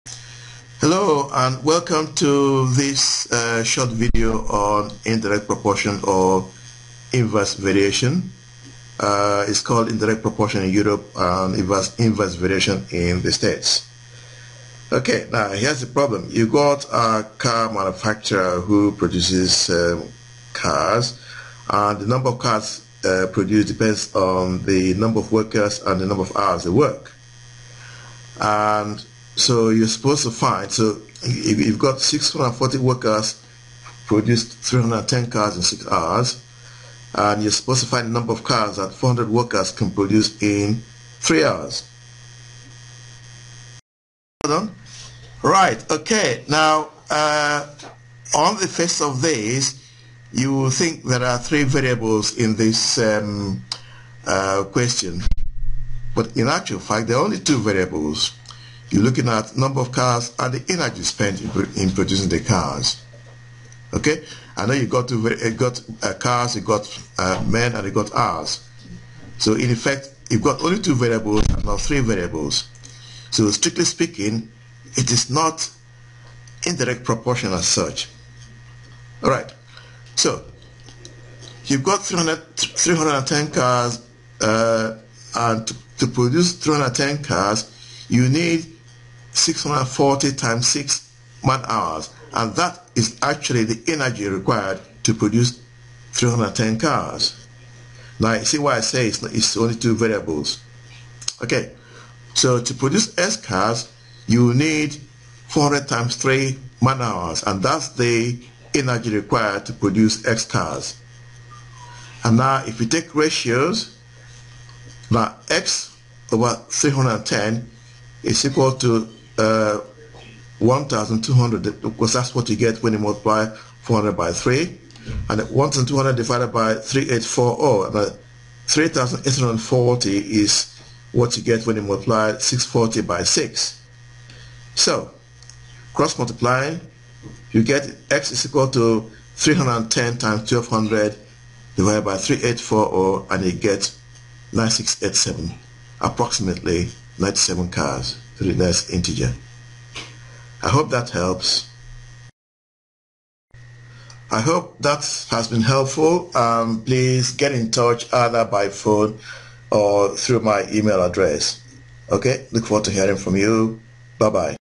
Indirect proportion, inverse variation, maths revision - IGCSE, GCSE, 8th Grade
I have produced this video to help you revise a special type ofinverse variation or indirect proportion. I have used the concept of man hours and a simple algebraic equation.

Is there a topic or maths question you'd like me to help solve?